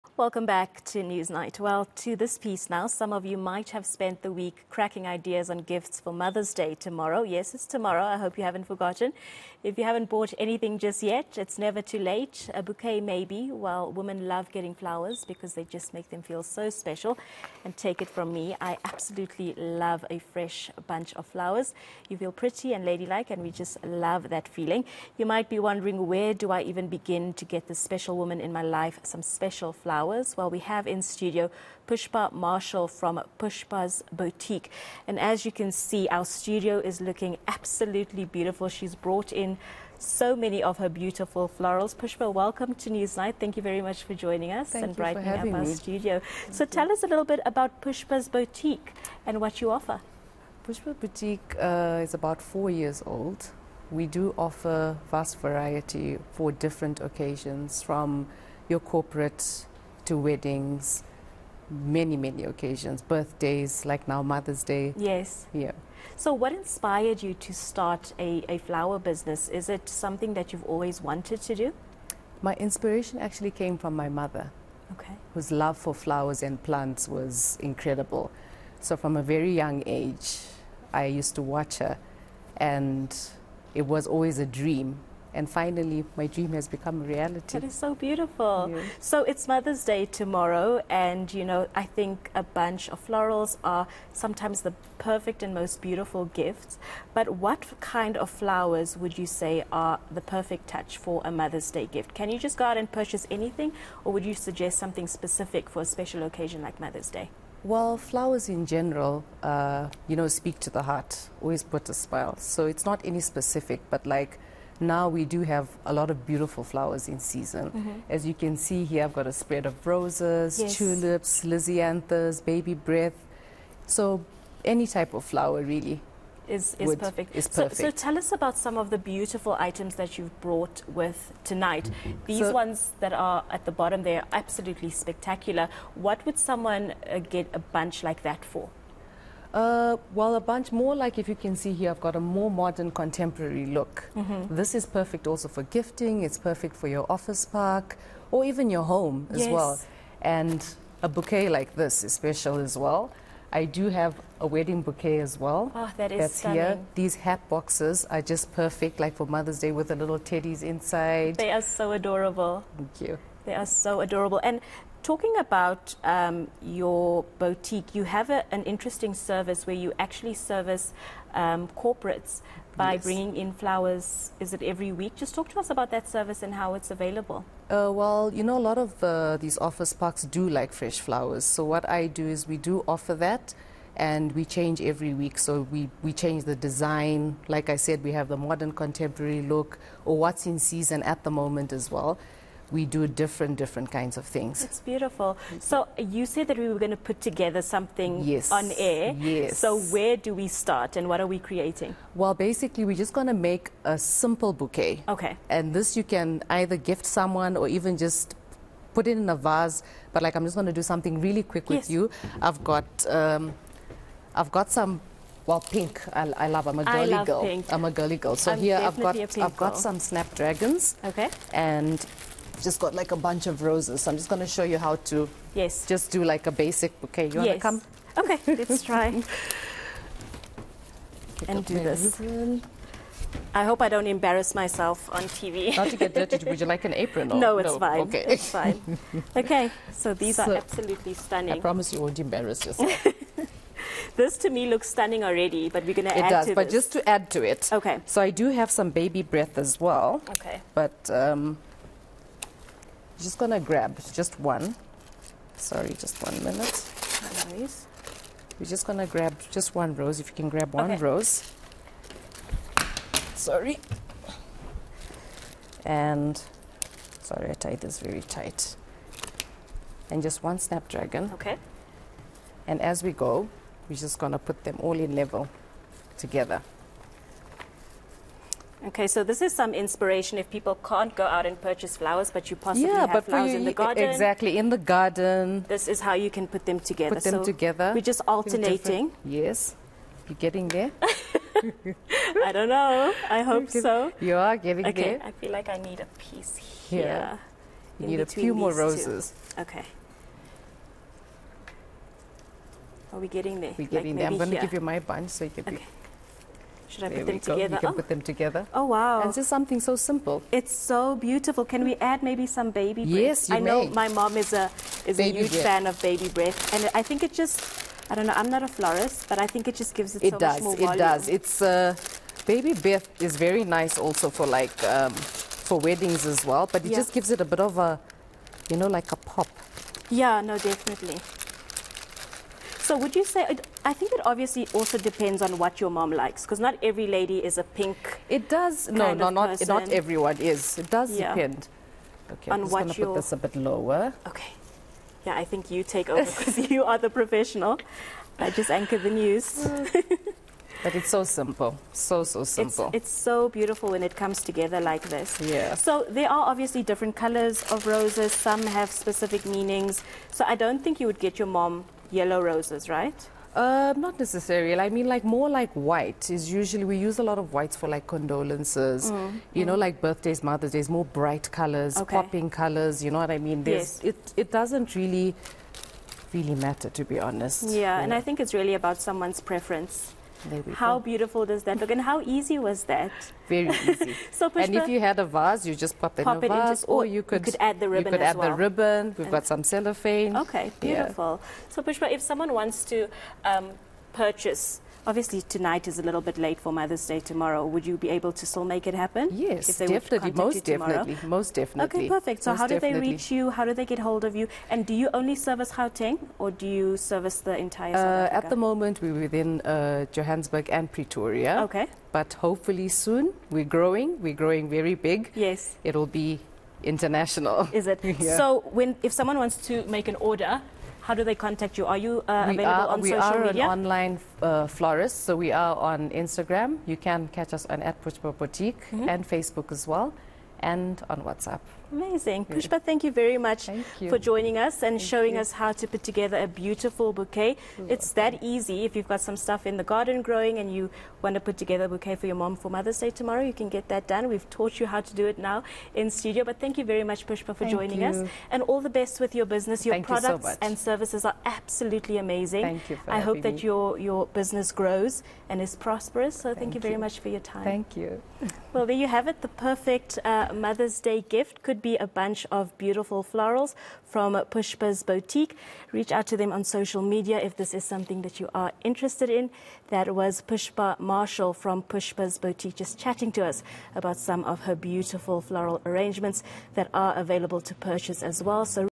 Exploring floral gift ideas
Some of you might have spent this week cracking ideas on gifts for Mother's Day. If you haven't bought anything just yet, it's never too late. eNCA speaks to Pushpa Marshall from Pushpa's boutique about different floral gift ideas that you could give to the special women in your life.